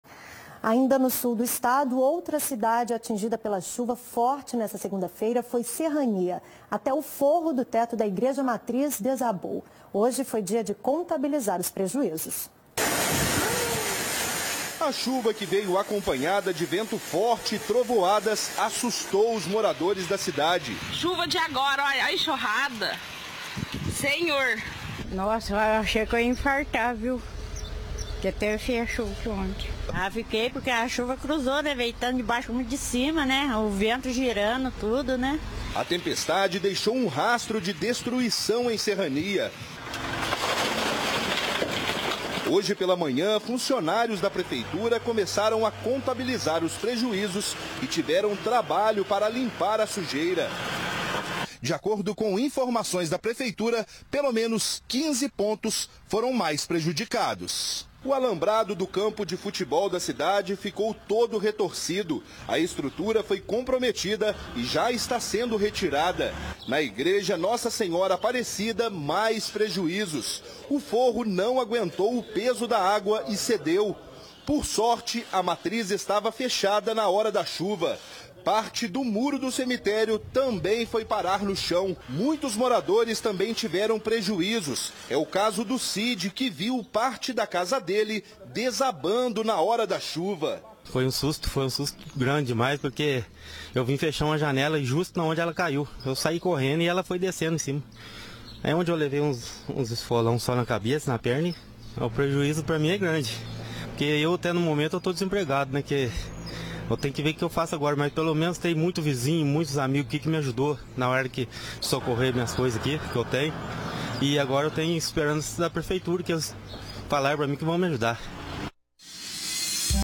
Chuva causa estragos em Serrania
Após o temporal, foi possível identificar os danos causados. Nem o treto da igreja matriz resistiu e acabou desabando.